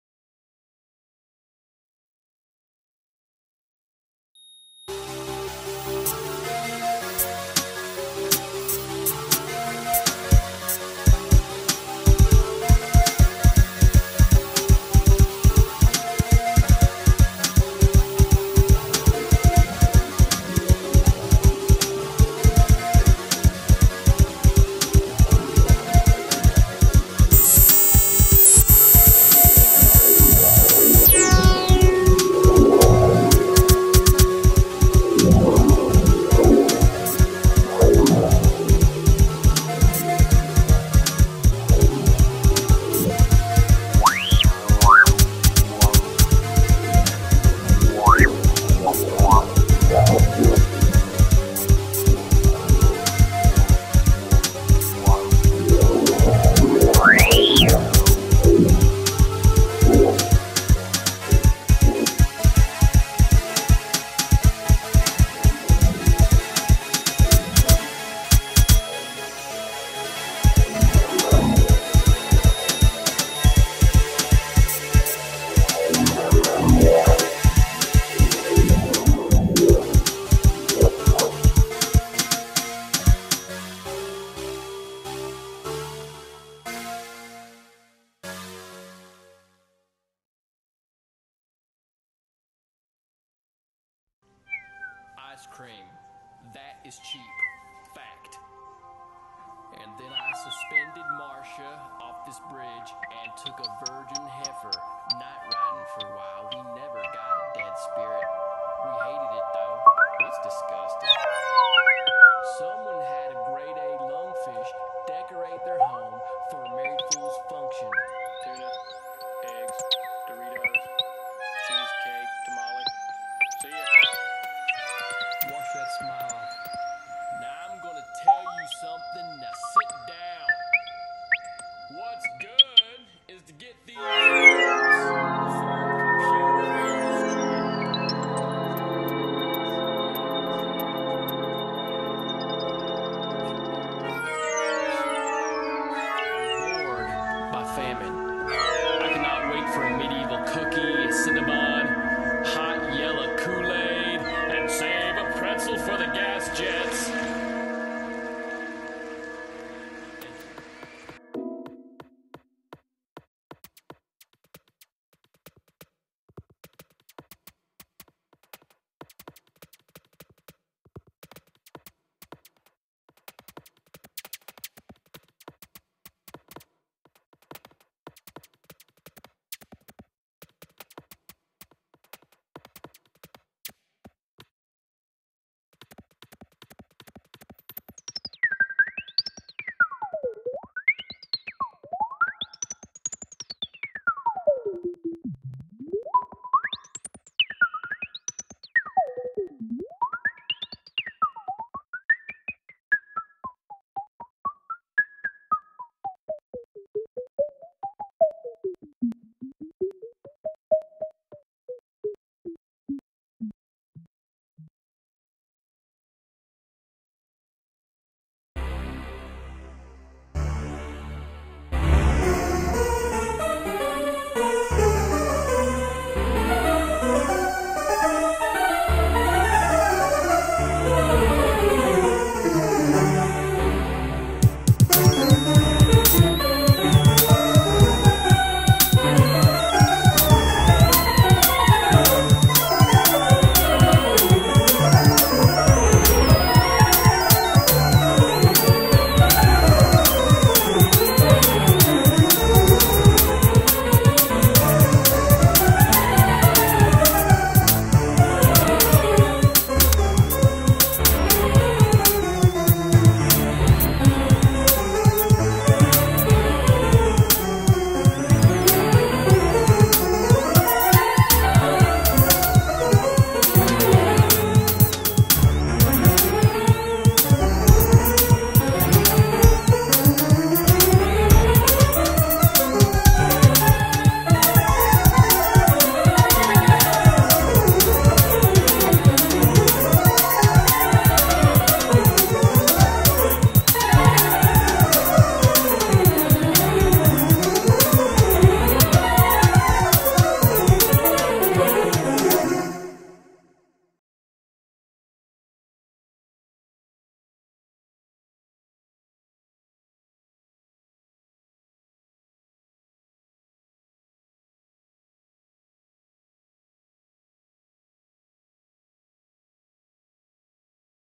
Save a Pretzel For The Gas Jets
Yo,

This was done for a music/multimedia class at SDSU- thought I'd share it since the music is so annoying... enjoy... shout out to BadLipReading for the awesome voice overdubs...

[BadLipReading is the shit. Check out the Youtube page.]

Everyone should know that Rick Perry could potentially be a bi-sexual swinger.  That would have been important information- assuming he lasted the entire primary season (which he didn't).  Either way, it's important to note that serious sociopaths are taking over this country (along with other sociopaths from outside this country), all in the name of Globalism.

-Fuck the U.N.

bUk FiDy